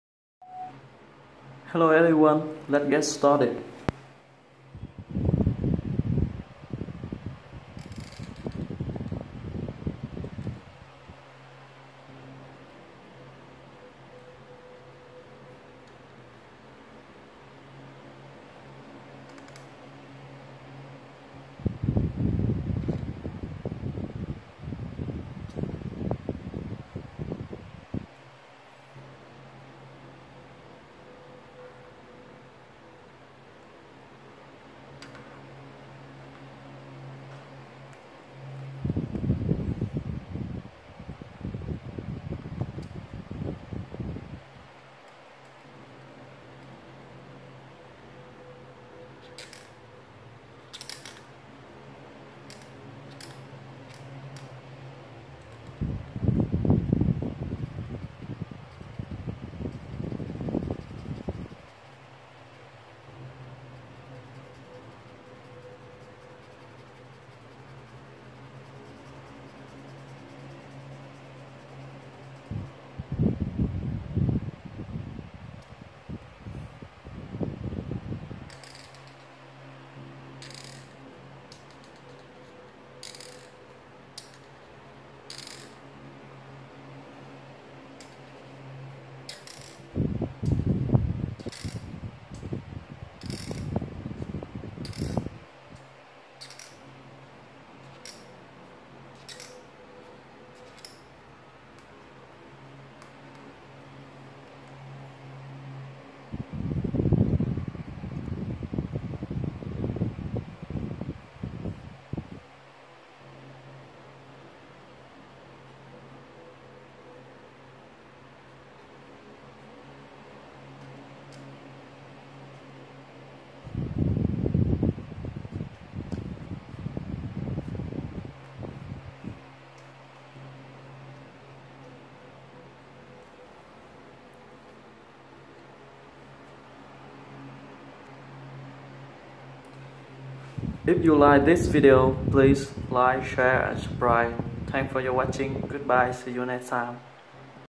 ART LESSON 62: HOW TO DRAW CARTOON Hedgehog STEP BY STEP - FOR BEGINNERS
ART LESSON 62: HOW TO DRAW CARTOON Hedgehog STEP BY STEP - FOR BEGINNERS EASY AND COOL, ALL YOU NEED TO DRAWING IS BLACK PEN AND WHITE PAGEPER, NOW LET'S GET IT STARTED !!!